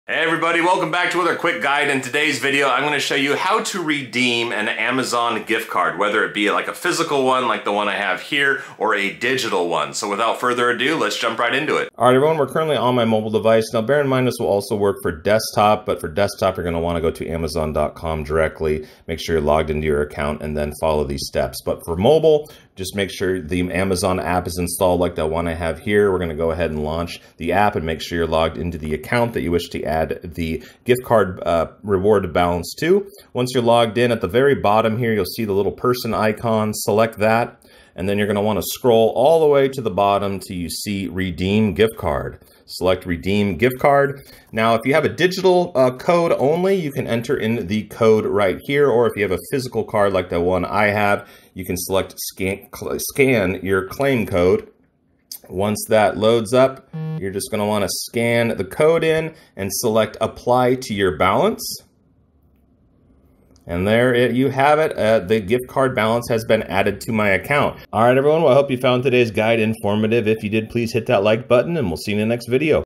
How To Use Amazon Gift Card (Redeem E Gift Card)
In today's guide I show you how to use Amazon Gift Card as well as how to redeem Amazon E Gift Card and finally how to claim Amazon e gift card.

If you are like me, you use Amazon for many of your purchases.  Perhaps you received an Amazon Gift Card from a friend or family member for your birthday gift or a holiday.  In this video, I show you how to redeem your gift card through the Amazon app although the same steps apply for desktop as well.  You can easily enter the gift card code manually if you only have a digital e gift card or you can scan it using your phones camera if you want it to be added automatically.  Once the Amazon gift card balance has been applied to your account, you can easily have it applied to your future purchases.  Or you can choose to save it for later.

Well everyone, I hope you found today's video helpful on how to use Amazon gift card and redeem Amazon gift card.